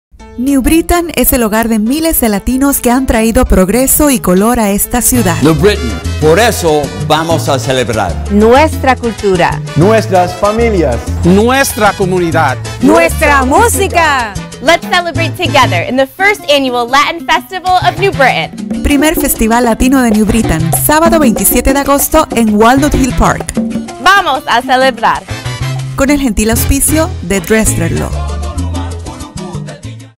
Latin Festival in New Britain - Aug. 27, 2016
The 1st Annual Latin Festival in New Britain - Saturday, August 27, 2016 @ Walnut Hill Park Dome.

Supported by: Dressler Law & Mega Education